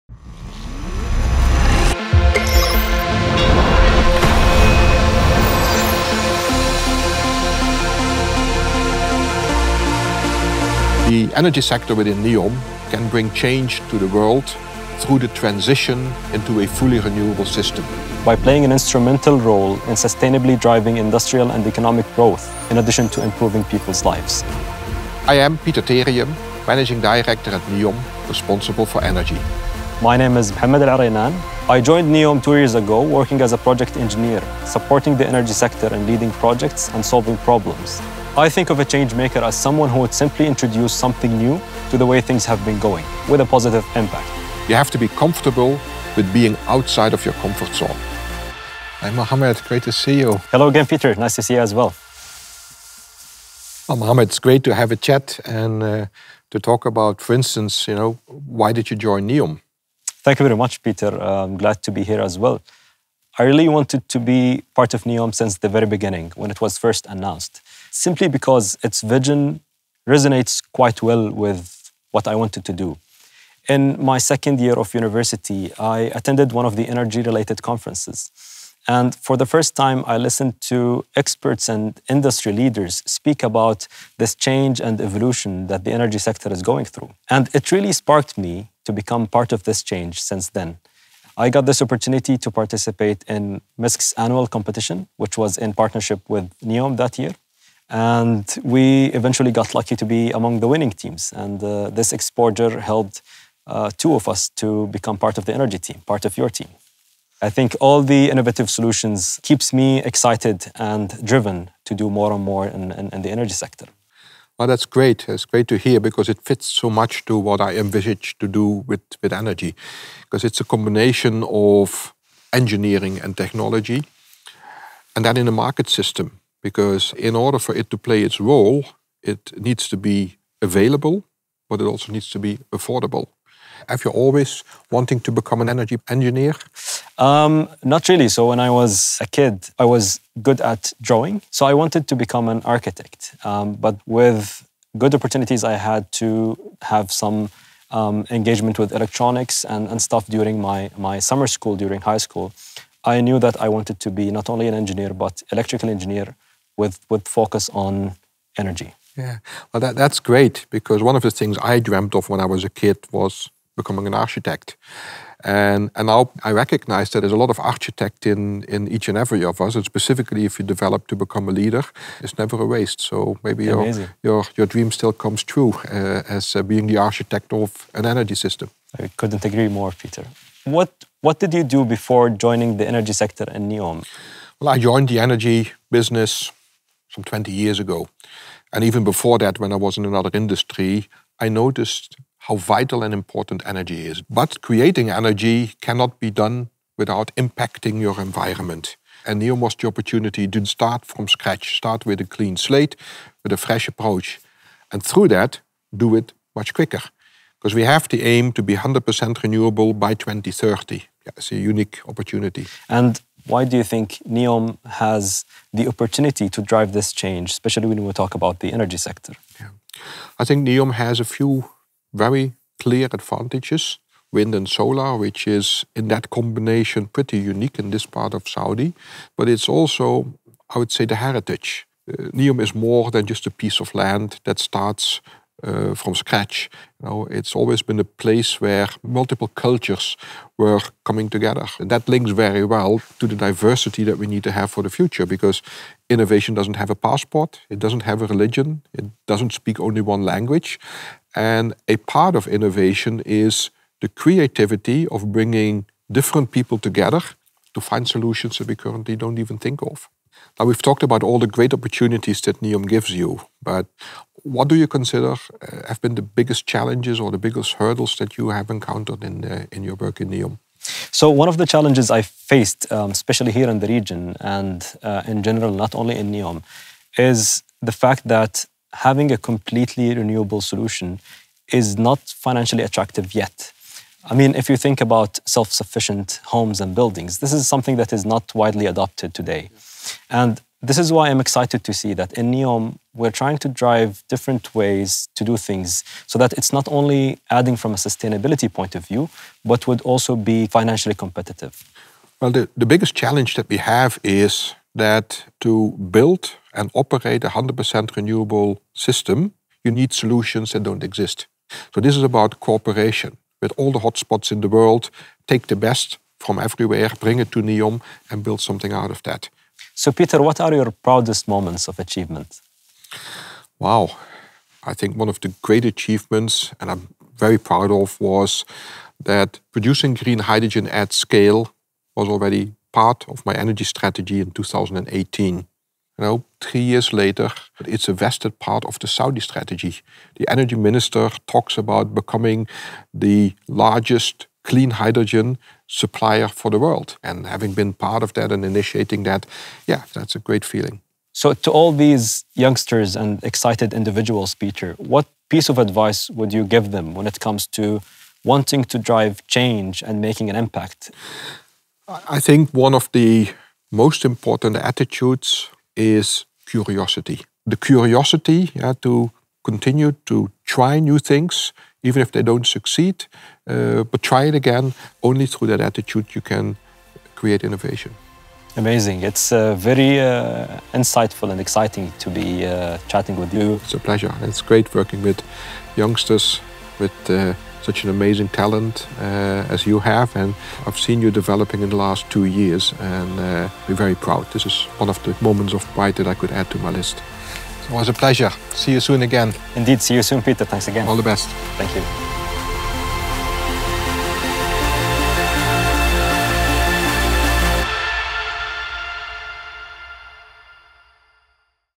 Changemakers Episode 5 – Moving beyond zero carbon to a circular economy
We are developing 100% clean energy at scale for NEOM and ultimately the entire planet.

NEOM Energy and Water CEO Peter Terium sits down with NEOM engineer Mohammed Aloraynan to talk about the challenges ahead of disrupting the energy industry and creating sustainability for all.
Watch the film to learn more about NEOM’s mission to drive change, while championing the young talent determined to make a positive impact across the world.

تعمل نيوم على إنتاج طاقة نظيفة بنسبة 100% على نطاق واسع ليس لأجل نيوم فحسب، بل لتشمل كامل كوكبنا.
يناقش بيتر تيريوم، رئيس قطاع الطاقة في نيوم، التحديات التي يواجهها قطاع الطاقة مع محمد العرينان، أحد المواهب الشابة في المملكة ومهندس مشاريع في نيوم. ويدور هذا النقاش الثريّ حول الأفكار والرؤى المتعلقة بأهمية تغيير النموذج المُتَّبَع في قطاع الطاقة بالانتقال إلى إنتاج واستخدام الطاقة النظيفة، وذلك لبناء مستقبل مستدام للجميع.
شاهد الفيديو لمعرفة دور نيوم المُلهم في صناعة التغيير وتشجيع المواهب الشابة التي تتطلع إلى إحداث التغيير الإيجابي في العالم لبناء مستقبلٍ أفضل.